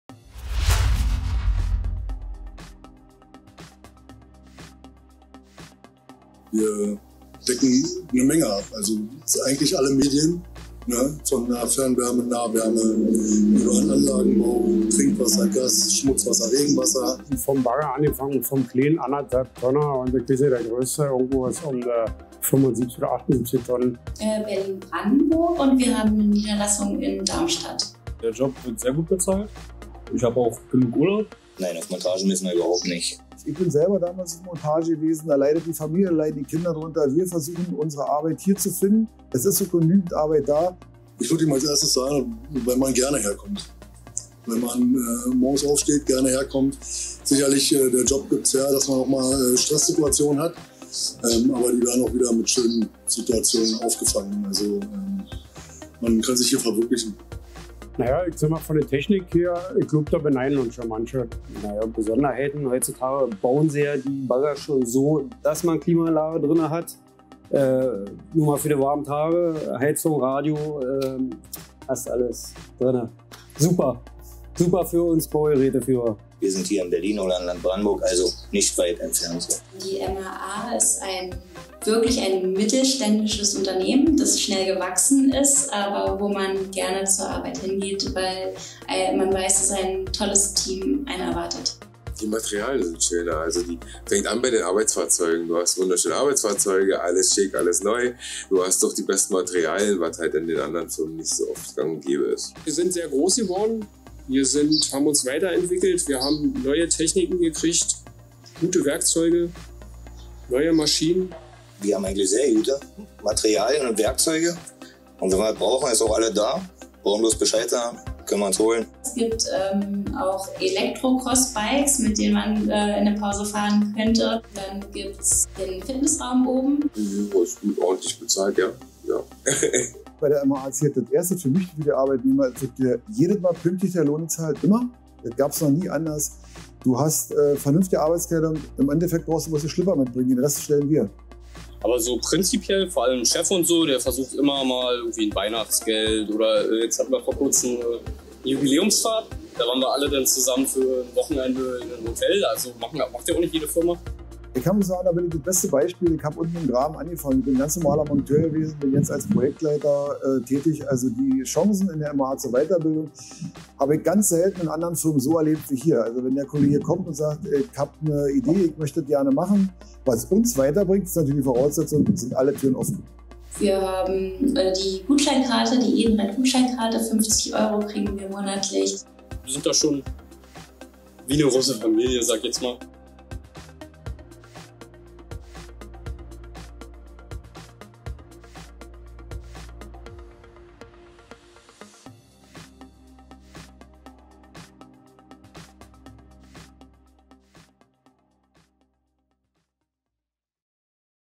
MRA Rohrleitungsbau Quadrat
Du bist Rohrleitungsbauer, Tiefbaufacharbeiter, Kanalbauer, Baugeräteführer, Schweißer, Selbstladerfahrer, Baugerätemechatroniker, Metallbauer oder Anlagenmechaniker und suchst Veränderungen? Dann bist Du bei uns bei der MRA herzlich willkommen. Bewirb Dich hier in nur 3 Minuten und finde heraus, ob ein Job bei uns das richtige für Dich ist.